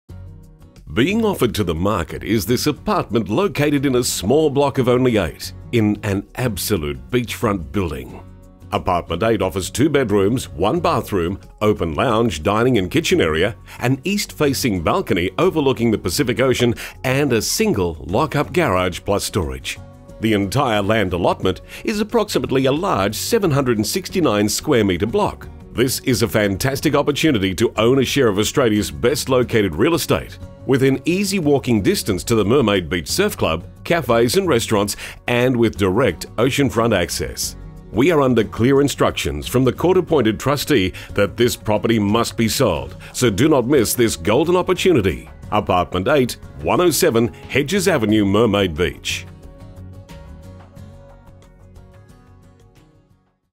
8/107 Hedges Avenue, Mermaid Beach QLD By Michael Willems
Property Video shoot of 8/107 Hedges Avenue, Mermaid Beach QLD by PlatinumHD for Ray White Paradise Waters